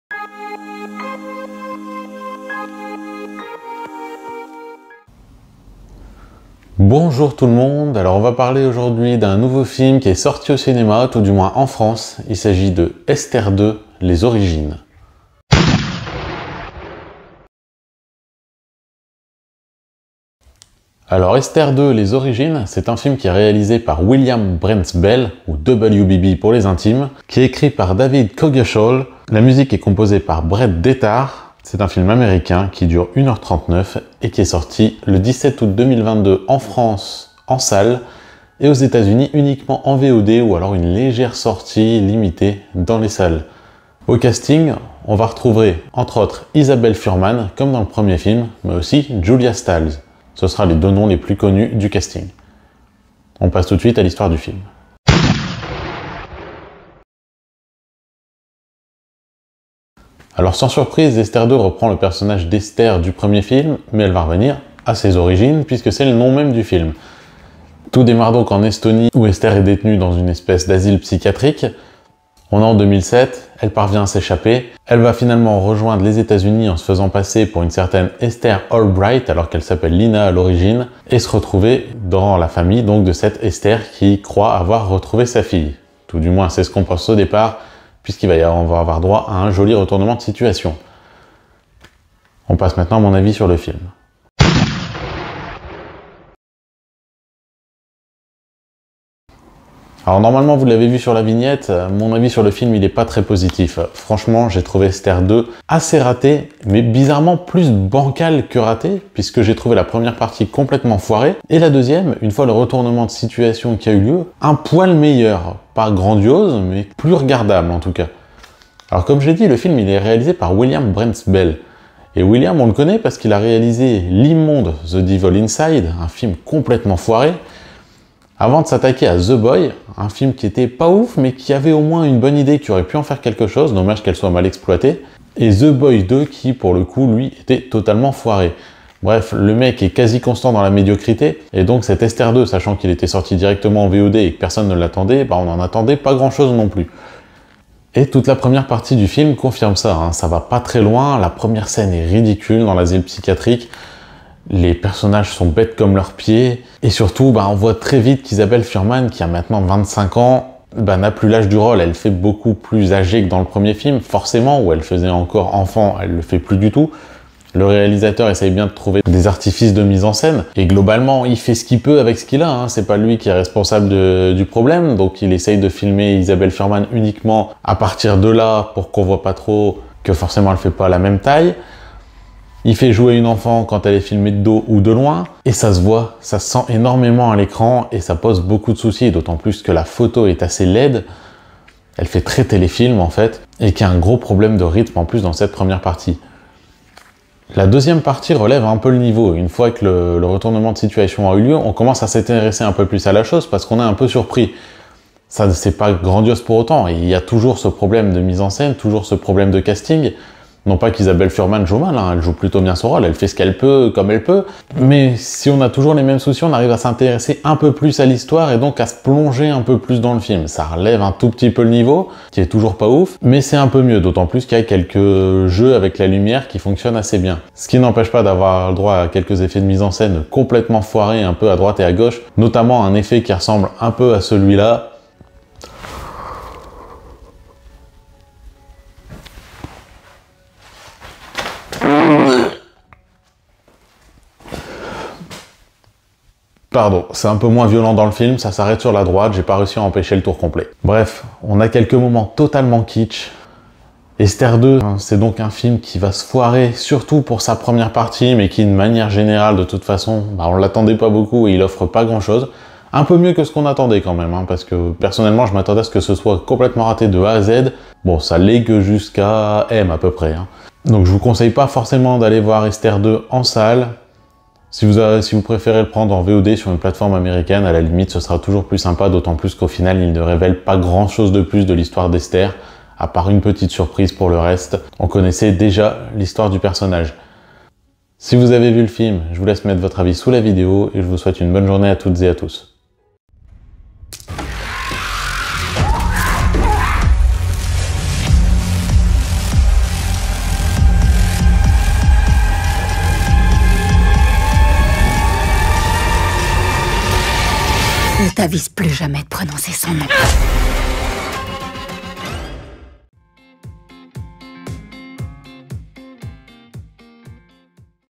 Cinémoi #11 : Esther 2 : Les origines
Mon avis sur Esther 2, un prequel dont on se serait bien passé !

Esther 2 : Les origines, de William Brent Bell